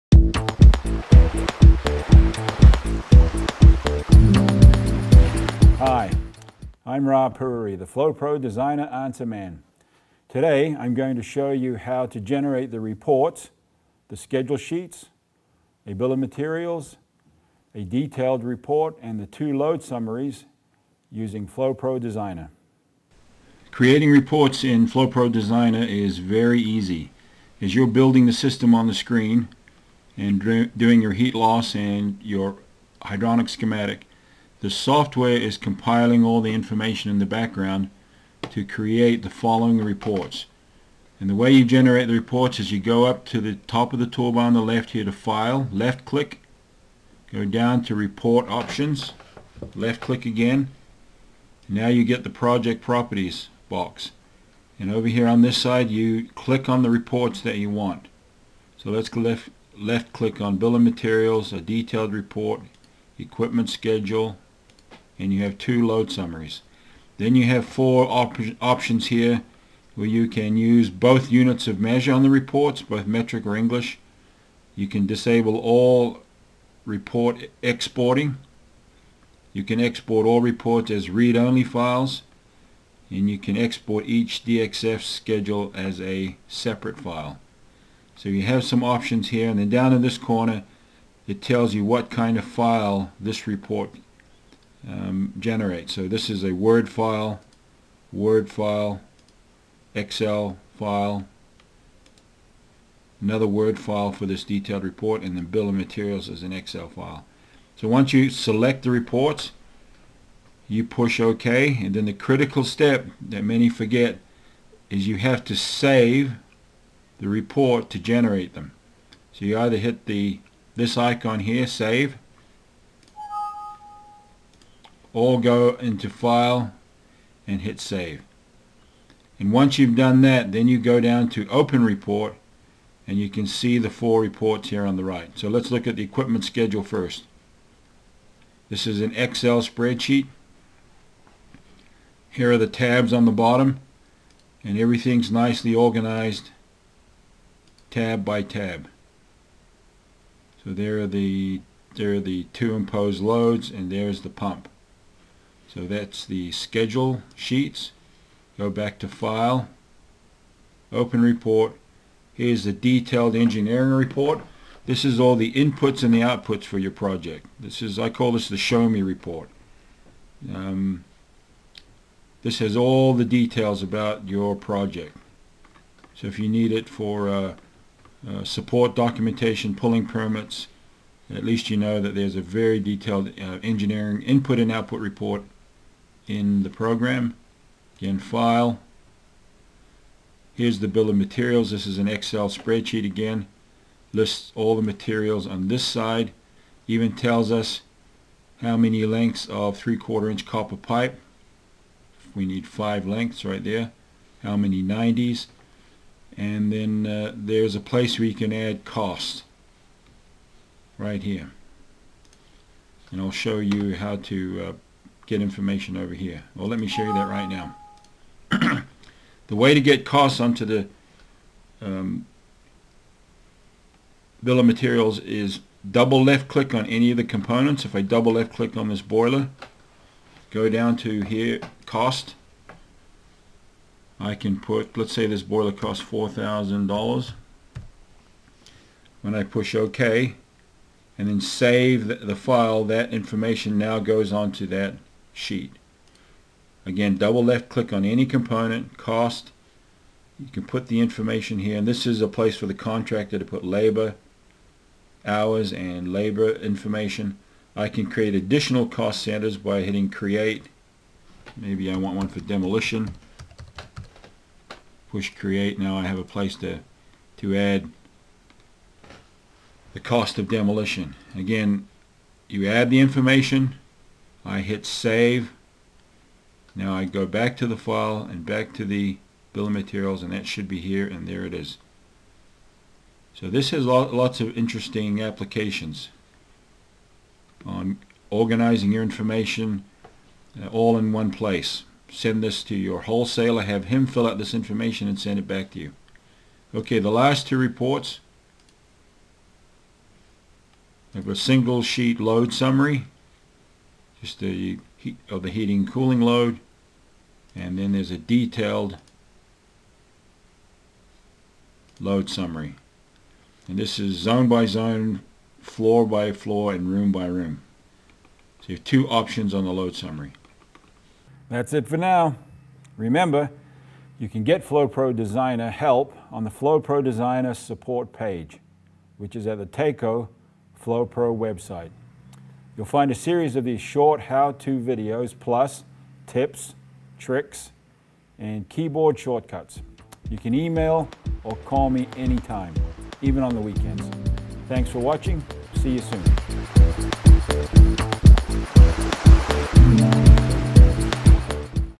Generating Reports in FloPro Designer
FloPro Team trainer Ra Puriri demonstrates how to generate and format reports in FloPro Designer including equipment schedules, bill of materials, load summaries and detailed reports.